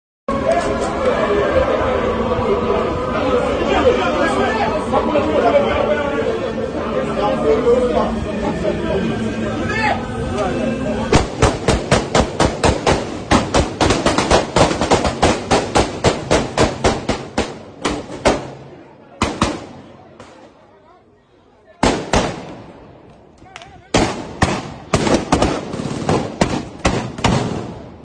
Premye minis Ariel Henry manke mouri gonayiv
Ayiti an kriz, Ayiti peyi spesyal, Gonaïves, indépendance d'Haïti